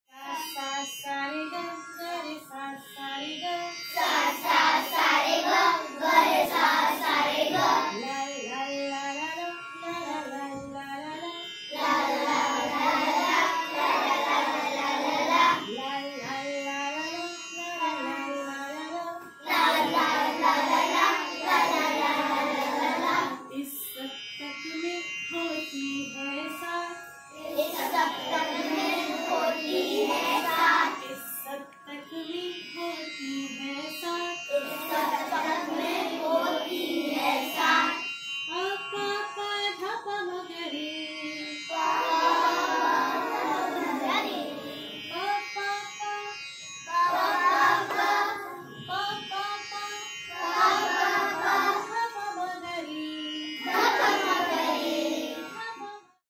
'Grow with Music' School Program | Shankar Mahadevan Academy
Students at C P Goenka Borivali learning 'Swara Mapping' with their teacher in class! 🎶

Get in touch with us to bring the 'Grow with Music' school program to your schools. 📚
Start your musical journey with Shankar Mahadevan Academy today.